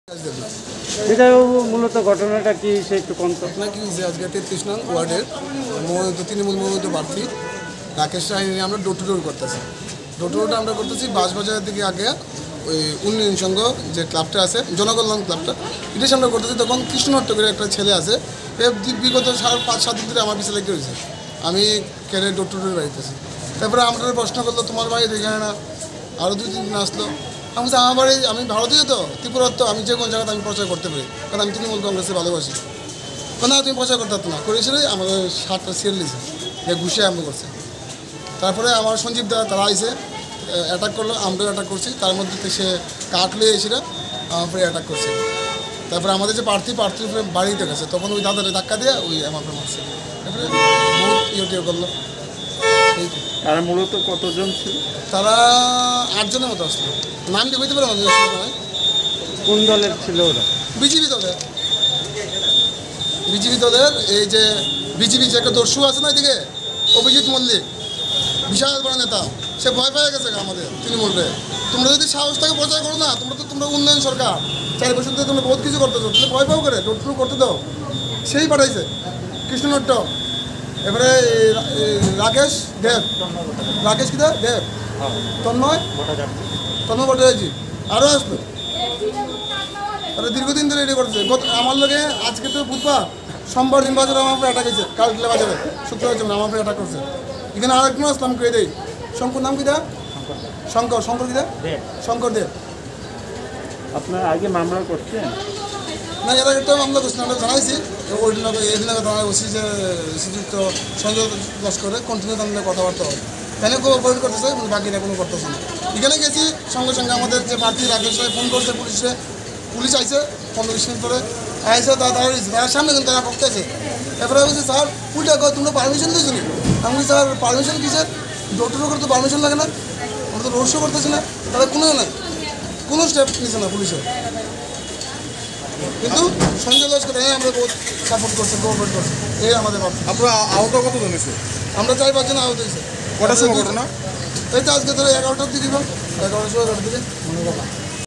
TMC worker Bikash Debnath beaten up by miscreant goons in AMC during a door to door campaigning
UPDATE_NOW || ফের আক্রান্ত তৃণমূল কংগ্রেস। এবার ৩৩নং ওয়ার্ডের তৃণমূল কংগ্রেসের প্রার্থী রাকেশ রায়ের সমর্থনে ভোট প্রচারে বেরিয়ে দুস্কৃতিকারিদের হাতে আক্রান্ত তৃণমূল কংগ্রেসের বেশকিছু কার্যকর্তা। গুরুতরভাবে আহত হয় বিকাশ দেবনাথ নামে এক কর্মী। উনার মাথায় আঘাত করা হয়, মাথায় তিনটি সিলি লাগে বলে জানা যায়। ঘটনা রাজধানী স্টিল ব্রিজ সংলগ্ন জনকল্যাণ ক্লাব এলাকায়। আহত বিকাশ দেবনাথকে চিকিৎসার জন্য আই জি এম হাসপাতালে নিয়ে আসা হয়।